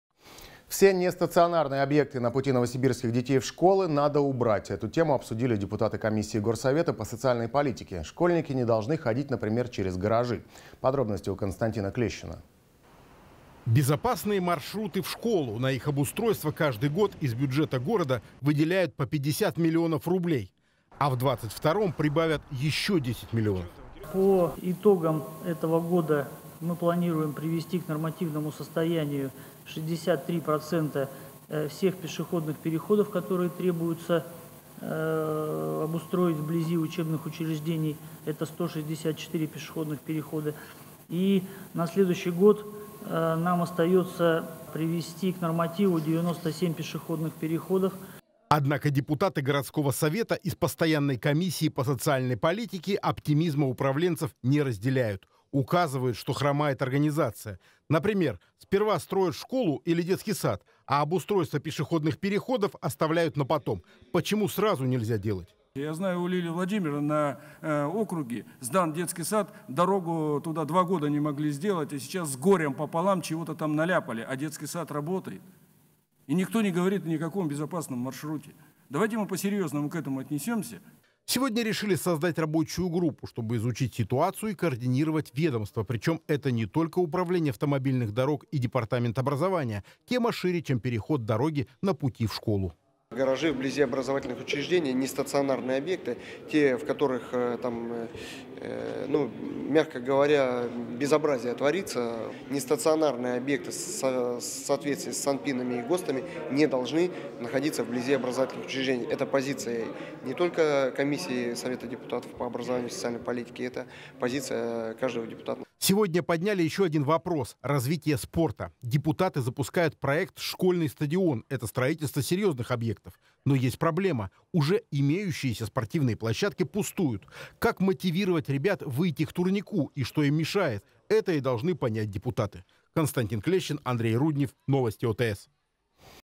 Депутаты горсовета Новосибирска обсудили безопасность маршрутов к образовательным учреждениям
Все нестационарные объекты на пути новосибирских детей в школы надо убрать. Эту тему обсудили депутаты комиссии горсовета по социальной политике. Школьники не должны ходить, например, через гаражи.